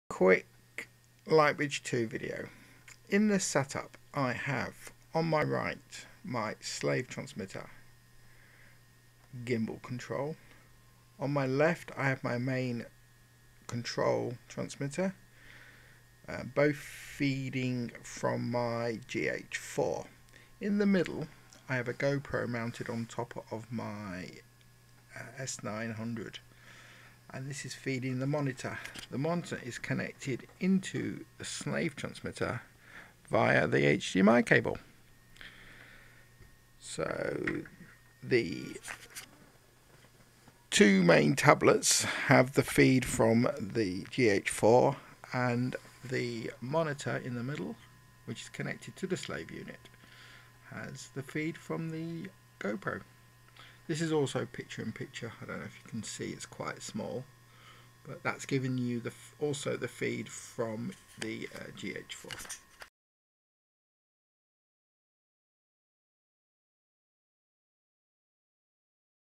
DJI Lightbridge 2. 2 Tablets 1 Monitor
In this setup I have 2  Lightbridge 2 ground station's one operates the controls the over operates the gimbel there is also 2 cameras the main one is on the gimbal the second is connected to the HDMI on the air system and feeds to a  FPV monitor connected to the HDMI on the slave ground station.